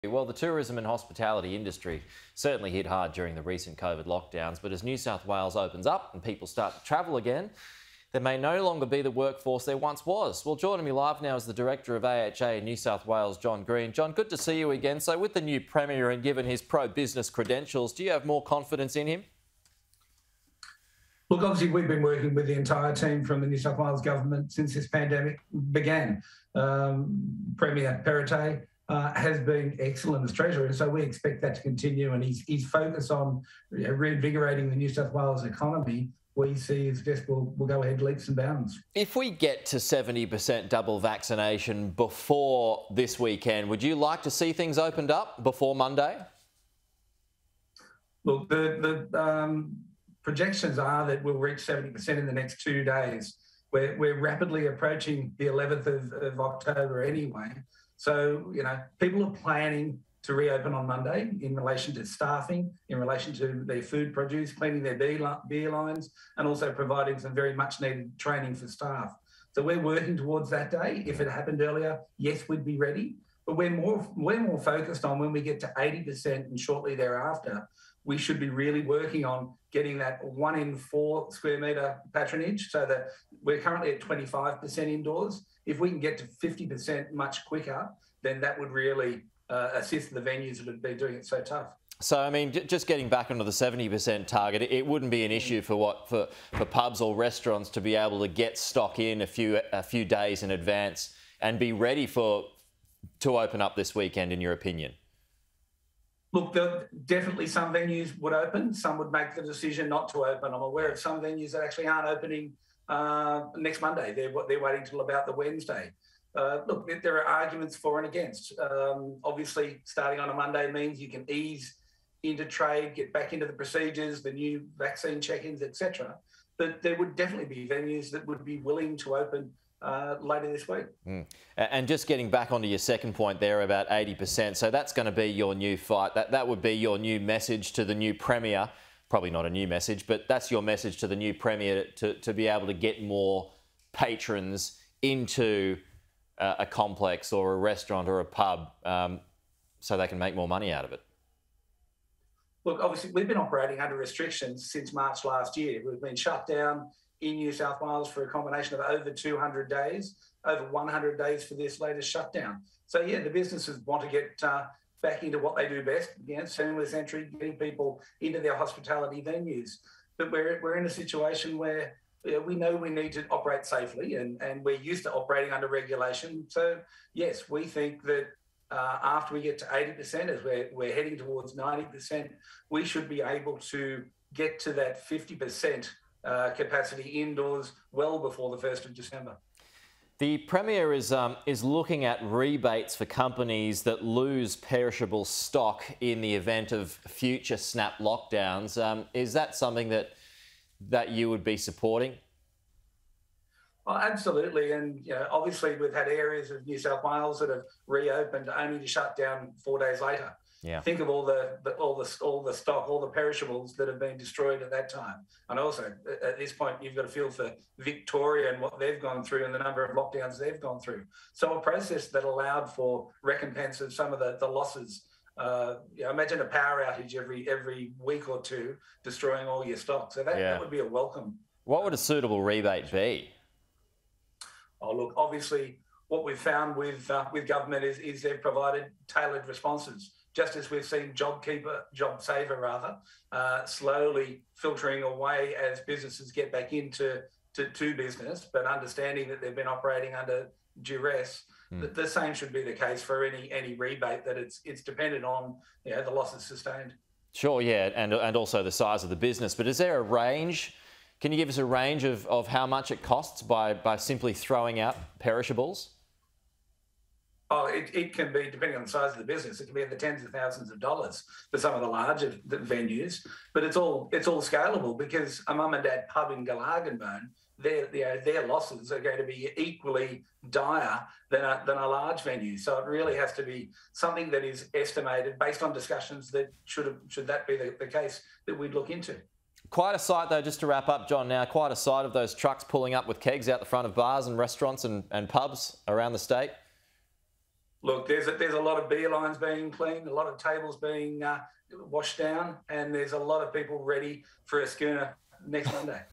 Perrottet's business-focused approach welcomed by NSW hotel association
Australian Hotels Association NSW Director John Green says he welcomes NSW Premier Dominic Perrottet’s focus on reviving the state’s economy.

“Obviously we’ve been working with the entire team from the New South Wales government since this pandemic began,” Mr Green told Sky News Australia.

"Premier Perrottet has been excellent as treasurer so we expect that to continue, and his focus on reinvigorating the New South Wales economy …  we’ll go ahead leaps and bounds."

He said if there were to be an earlier reopening than planned some businesses may welcome this, but many are preparing to open next Monday.

“The projections are that we’ll reach 70 per cent in the next two days, we’re rapidly approaching the 11th of October anyway.

“People are planning to reopen on Monday, in relation to staffing, in relation to their food produce, cleaning their beer lines and also providing some very much-needed training for staff.”